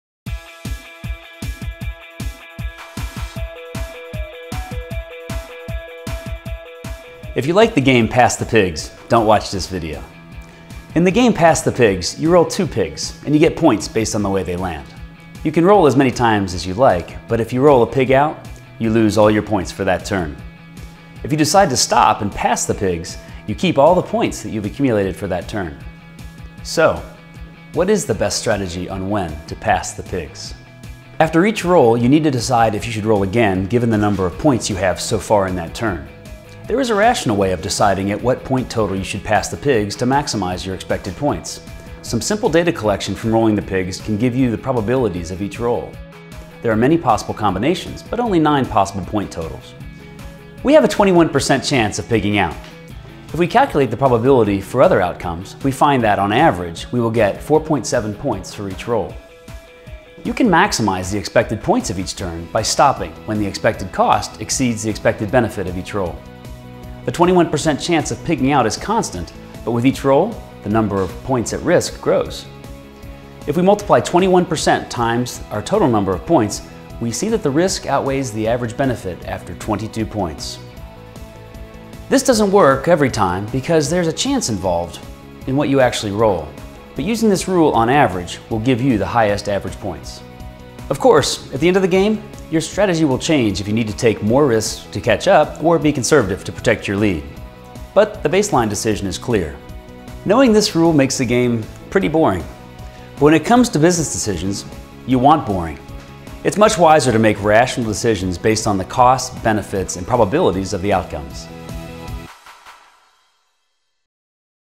How I Ruined "Pass The Pigs" for My Kids
Professor Michael Gorman, Ph.D. applies the principles of rational decision making to a kids game to determine the optimal strategy.